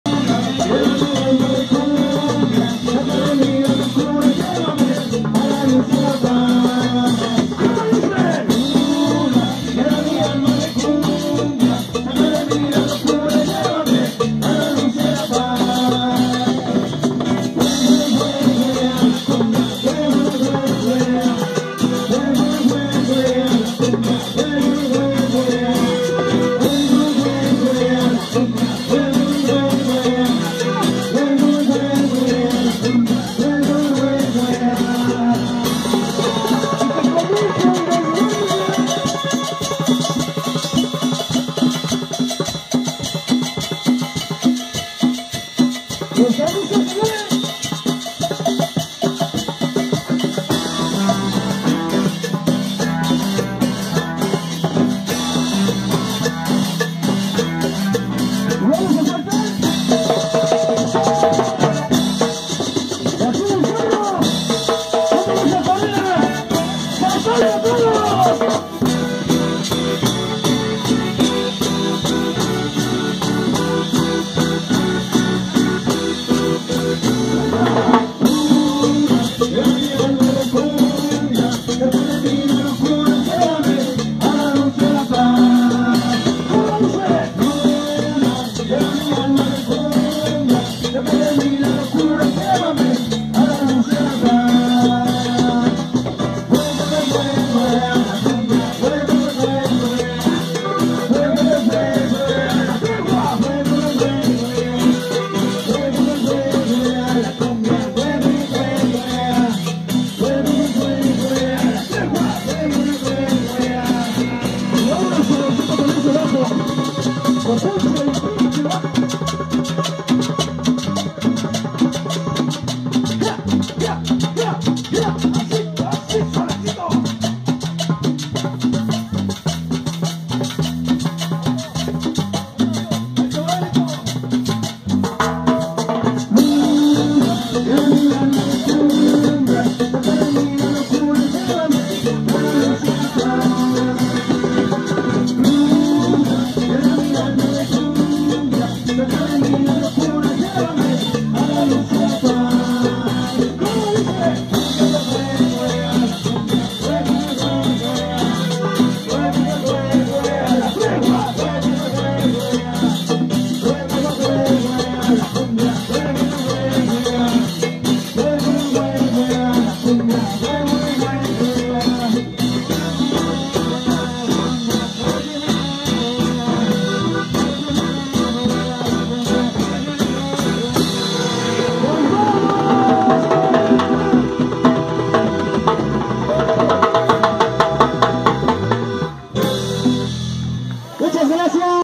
SUPER GRUPAZO LA TREGUA - LAS CRUCES
Esta vez la tregua se fue a  el cerro ahí con los amigos de corena en camino viejo a Chalma muy buen ambiente muchas felicidades y bendiciones para los trabajadores de corena